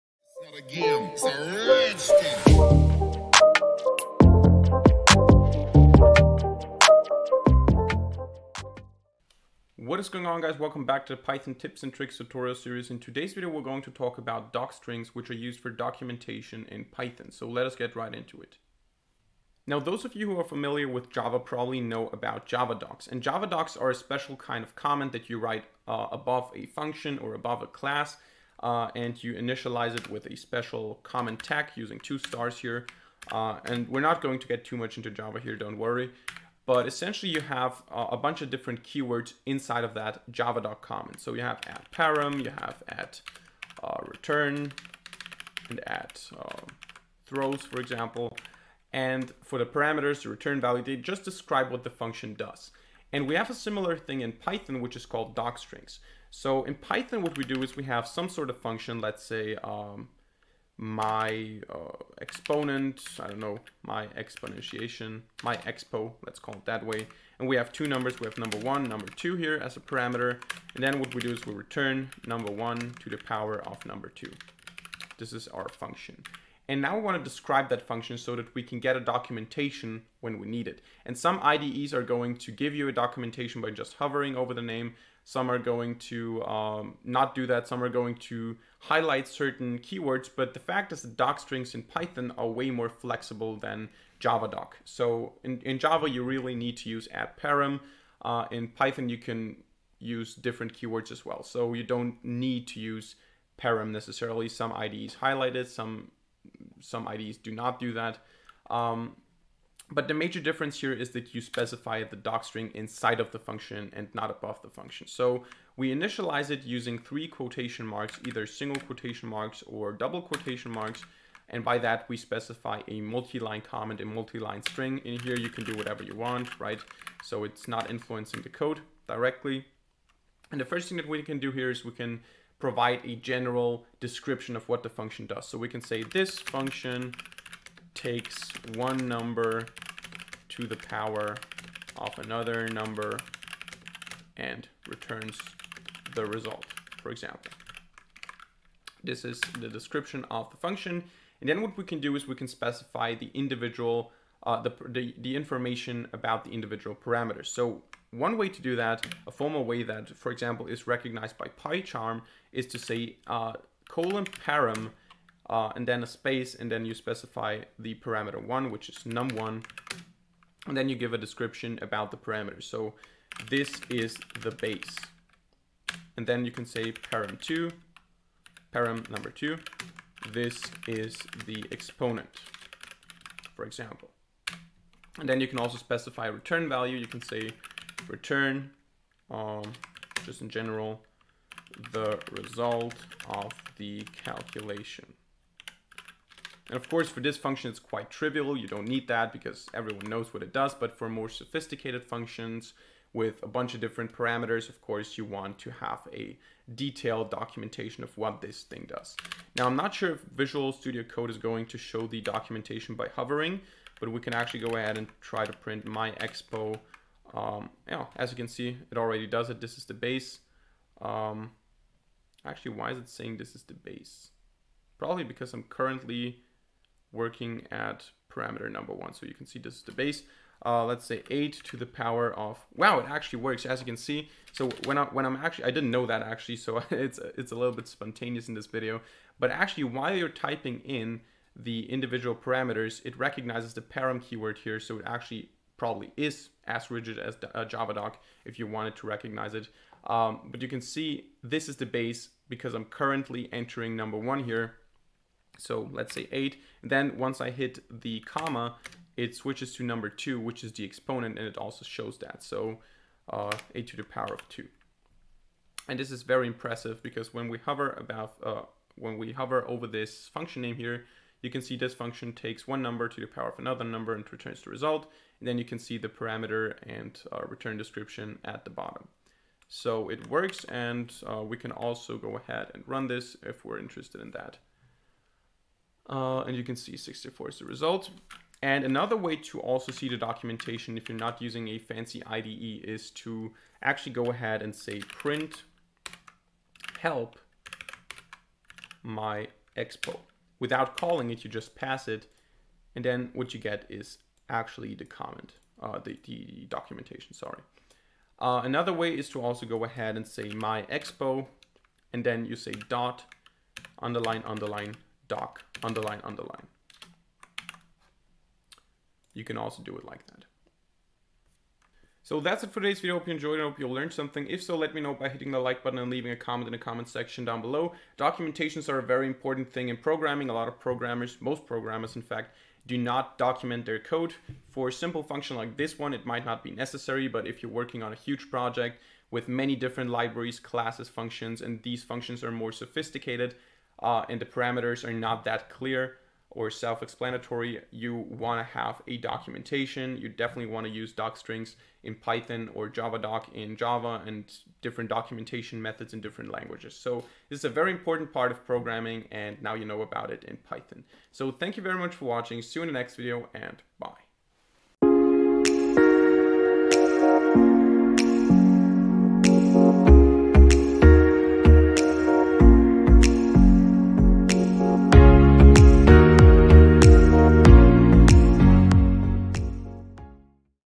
Docstrings - Python Tips and Tricks #10
Documenting your code in Python is super important for larger projects. Today we learn how to do it properly!